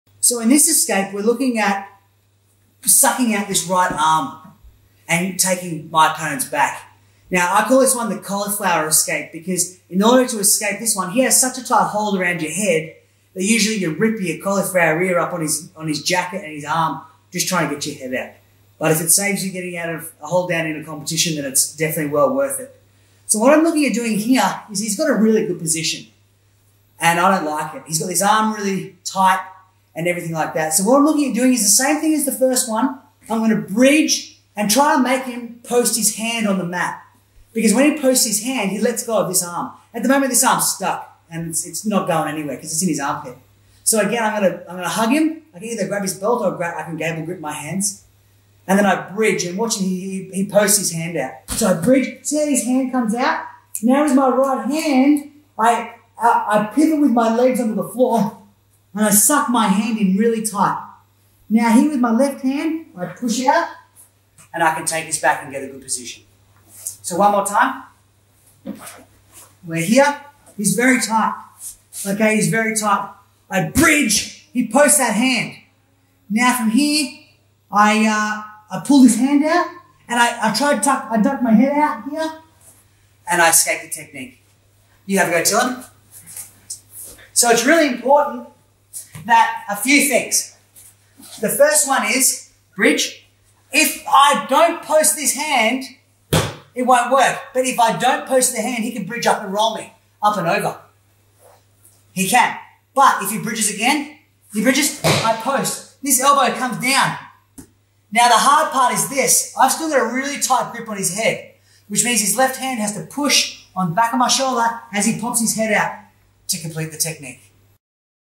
Kesa Gatame Cauliflower Escape by Matt D'Aquino
KESA GATAME CAULIFLOWER ESCAPE

In this Judo training video, Matt D'Aquino teaches the Kesa Gatame Cauliflower Escape technique.

Matt D'Aquino is a renowned Australian judoka who represented his home country in the 2008 Summer Olympics.  This is an excerpt from LIGHTNING FAST ESCAPES FOR COMPETITION JUDO available from JudoFanatics.com.